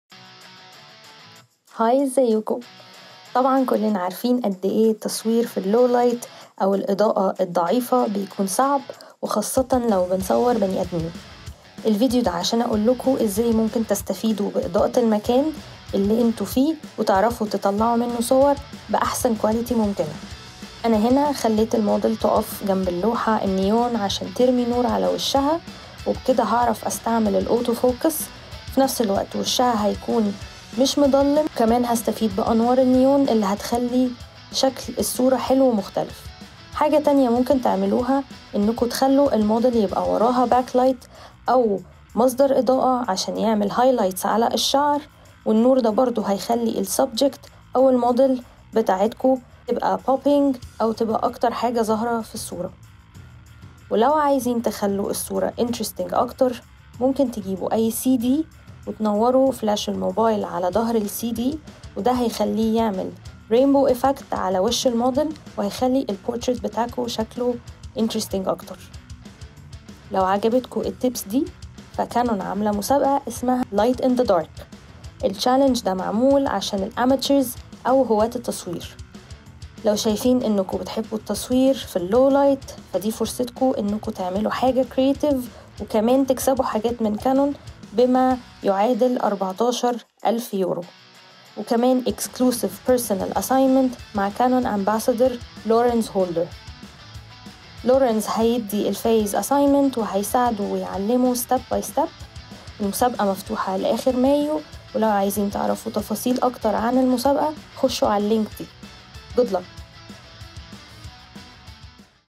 Pushing the Creative Limit with Menna Hossam
Get tips from Canon Ambassador Menna Hossam, who shows how to tackle creative possibilities of low-light shooting.